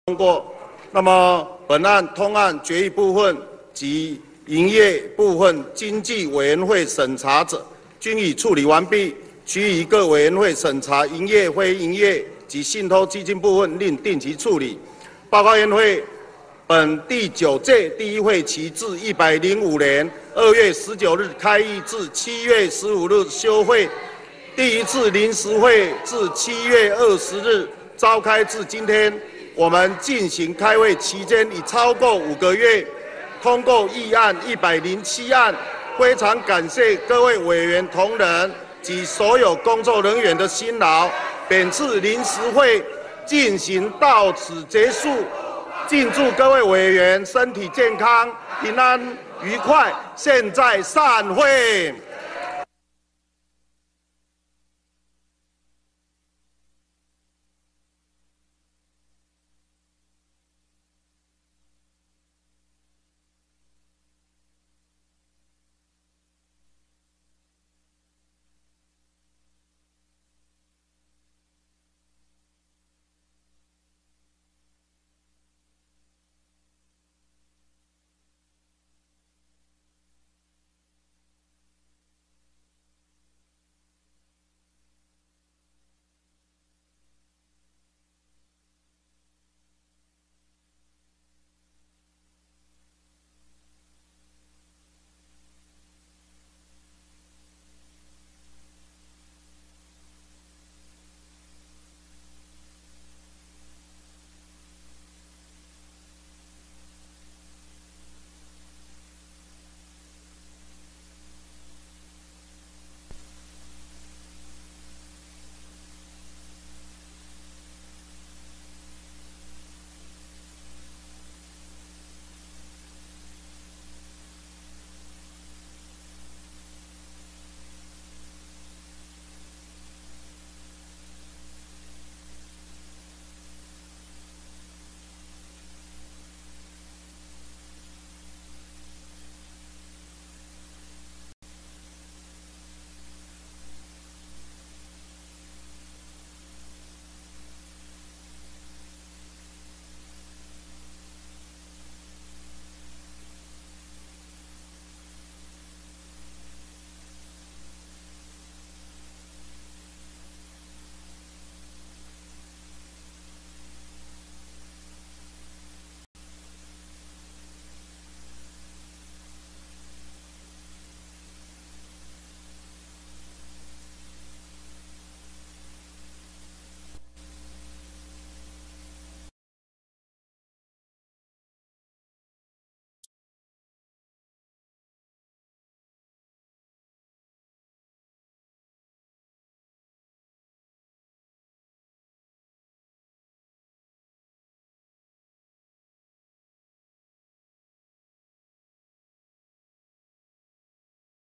20160728 院會 臨時會 PM1750 蘇嘉全 宣佈臨時會結束 【立法院演哪齣？】
審到台水
終於演完了...orz